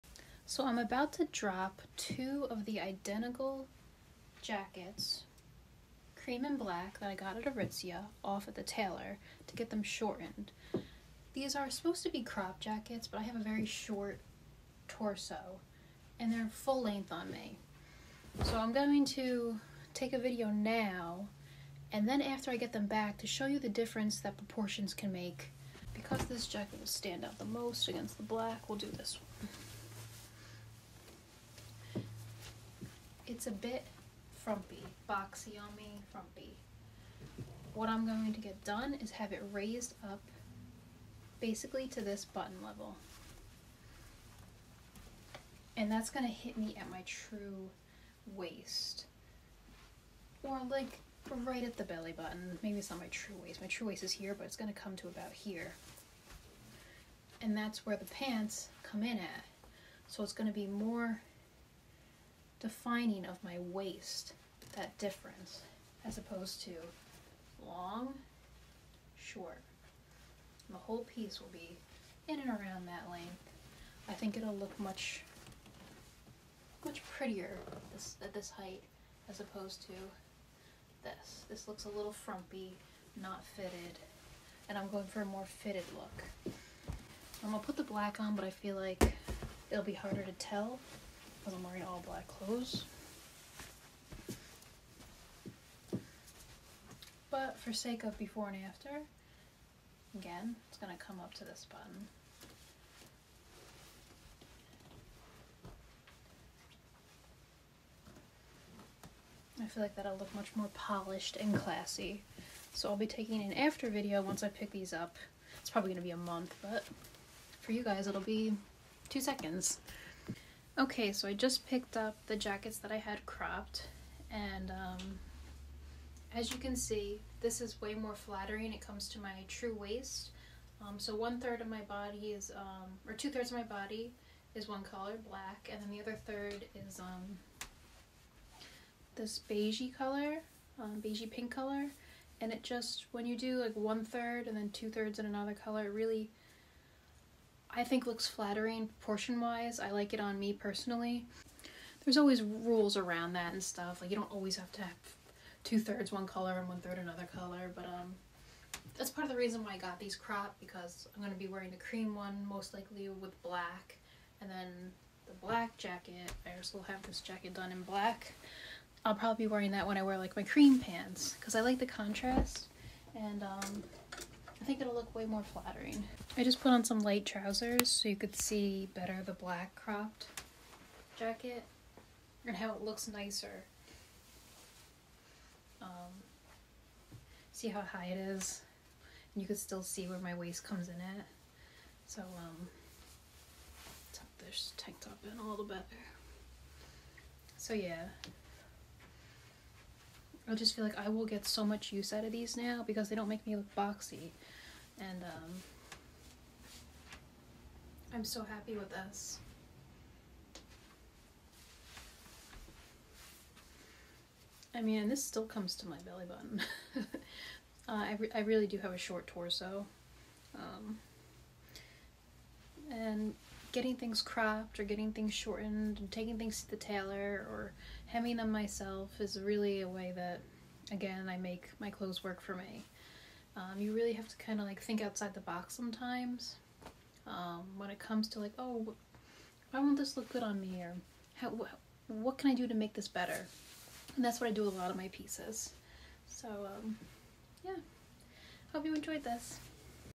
Tailoring your Clothes can Completely Change Your Figure | Aesthetics of Olive
I got these two Aritzia shirt/jackets shortened. The shorter proportions works so much better for my figure. I have a short torso and these are much more flattering on me now.

• Jackets: Aritzia -  Wilfred Free, Cropped Ganna Shirt Jackets (size S)